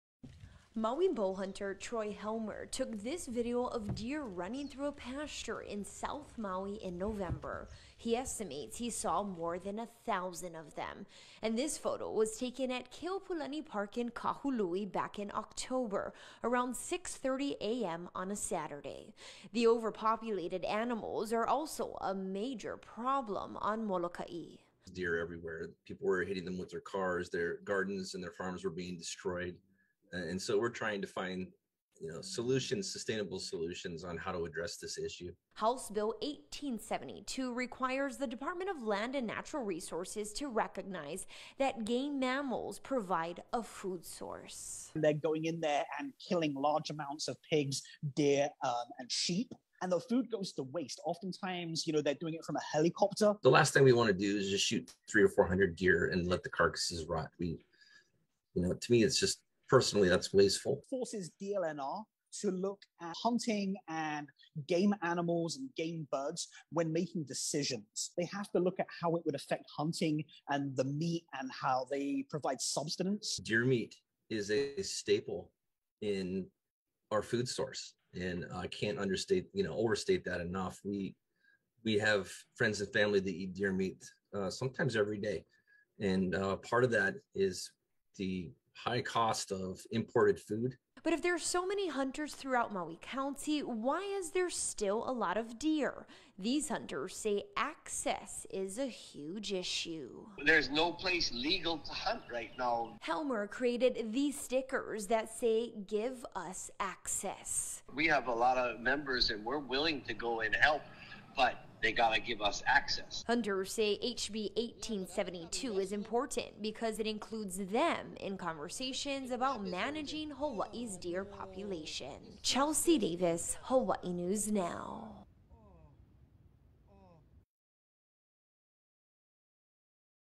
"Give us access."Maui County deer problem" Hawaii News Now
///@chelseadavishnn: "Give us access."

Hunters want to help with Maui County deer overpopulation.

More on @hawaiinewsnow.